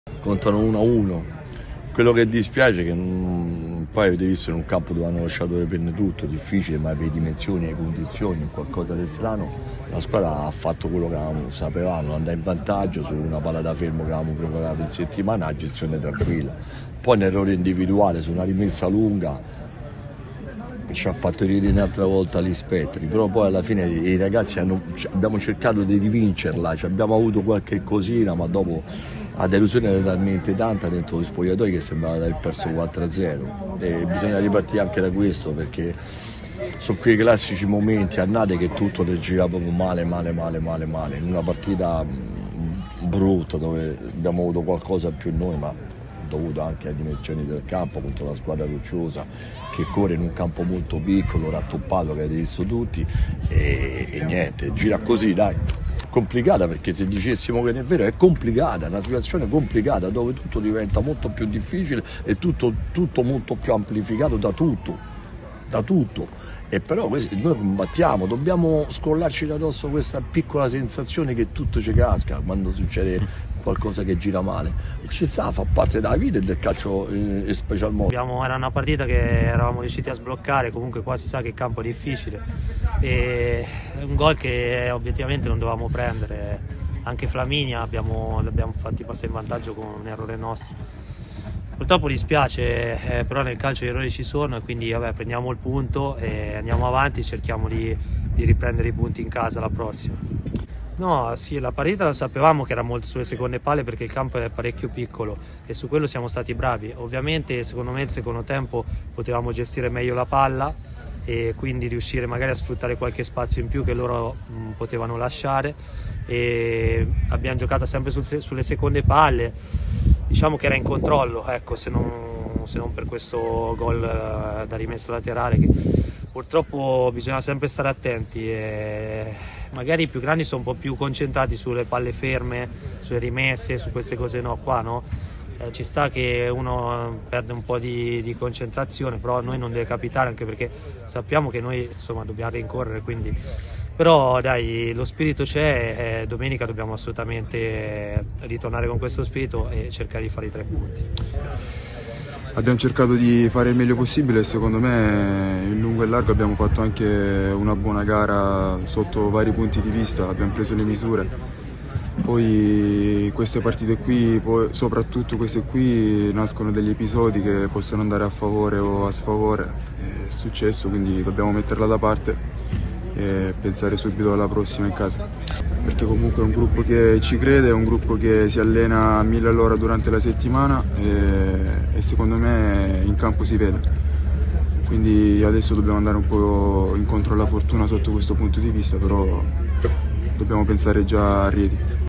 Ennesima occasione persa per l'Arezzo, pareggio in trasferta col Montespaccato
Al "Don Pino Puglisi" gli amaranto sono passati in vantaggio al 28' del primo tempo con Marchi ma si sono fatti riprendere sul finale su errore di Mastino. Un pareggio che non da continuità alla vittoria di domenica scorsa con l'Unipomezia e che lascia l'amaro in bocca.
Le parole di Mister Mariotti.